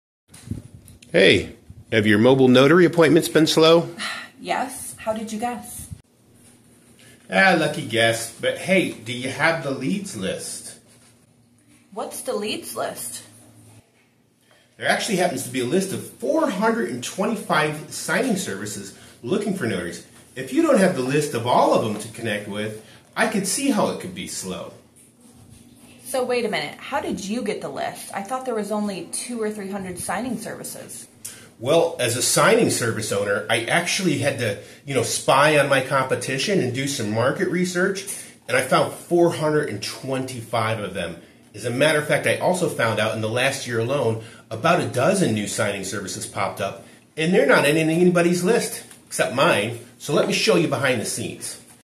Steal My Secret List of Signing Services To Get More Notary Jobs on Autopilot
Get more notary jobs & appointments with the most comprehensive list of signing services & notary agencies available on the market. Prices for all budgets to get you making $$$asap! Get the lists here: